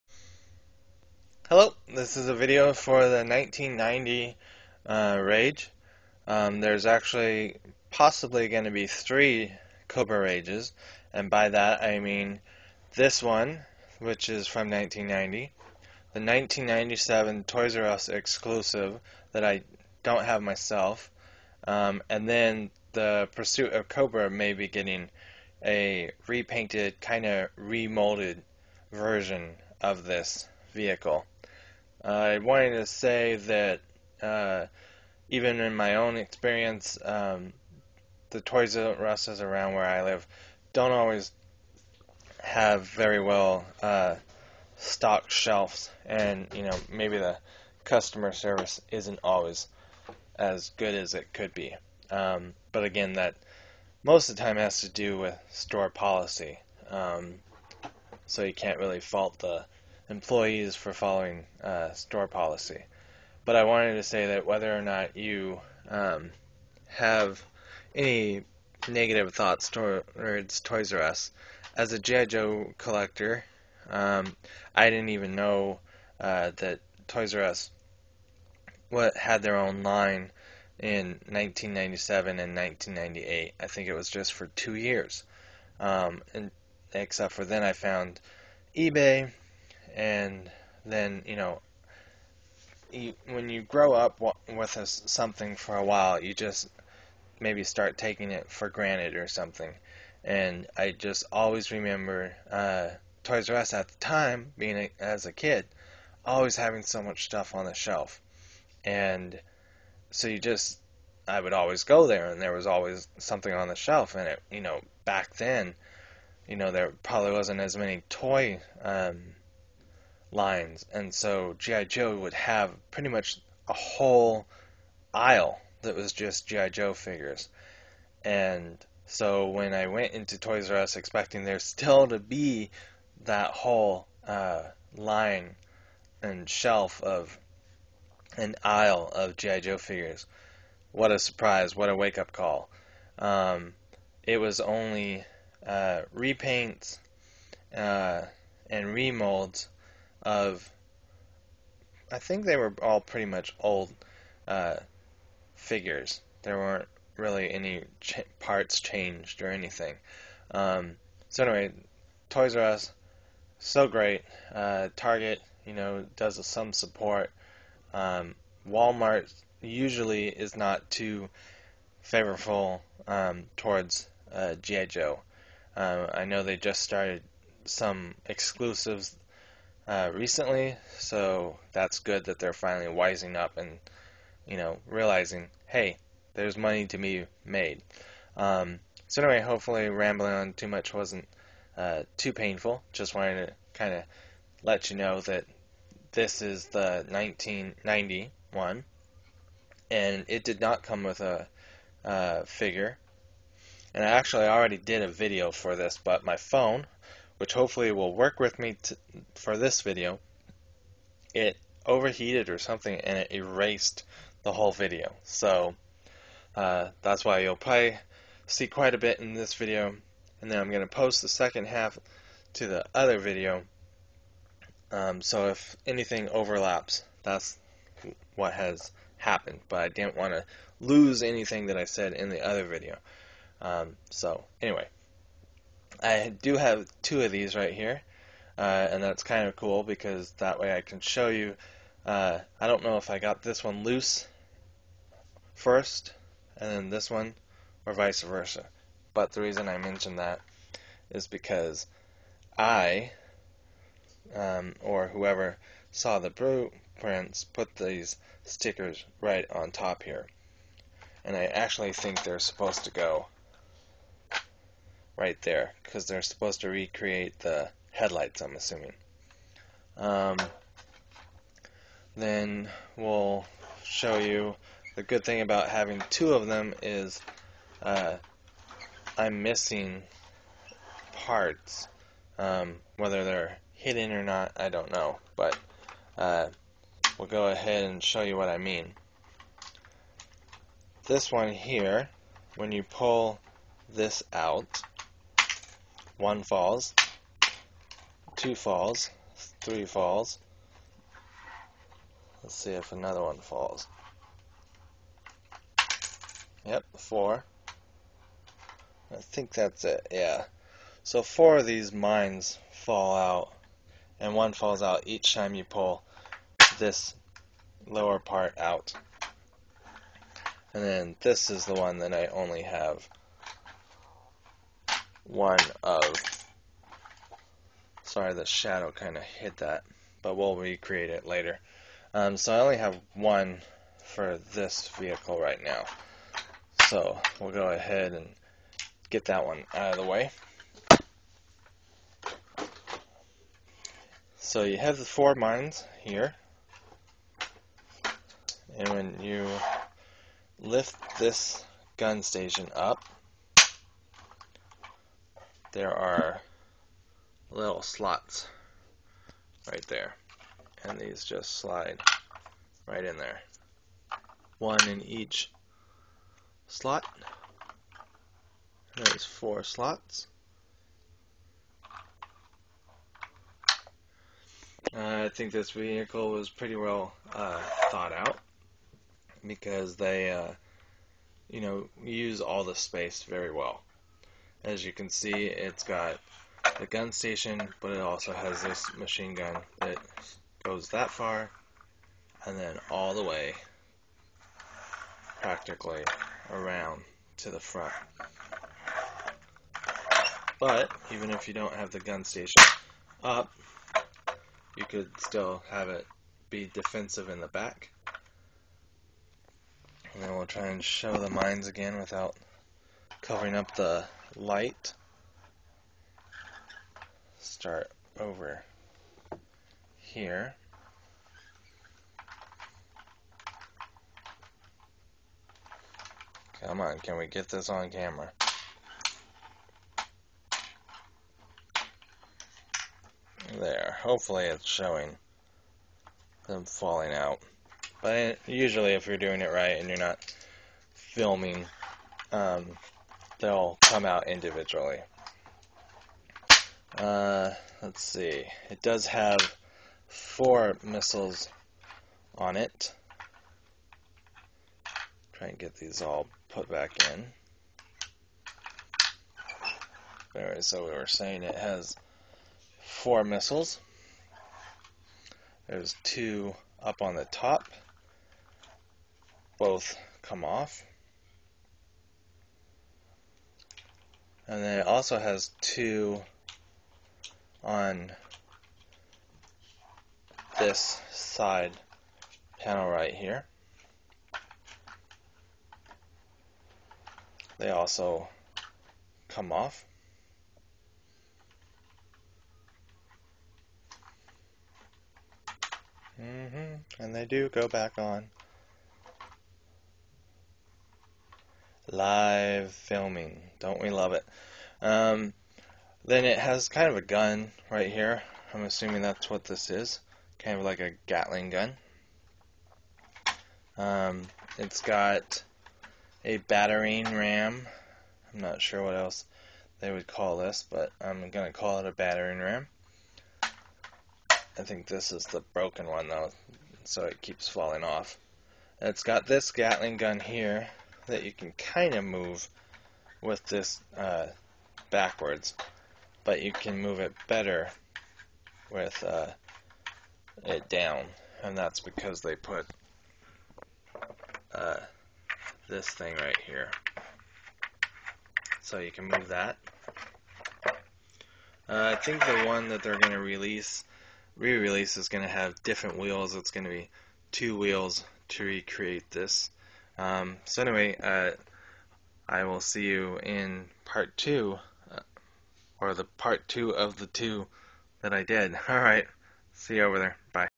G.I. Joe 1990 Cobra Rage A redo for my first part1 that got deleted.3gp
My first part one was deleted by phone I tried to recreate my part 1:(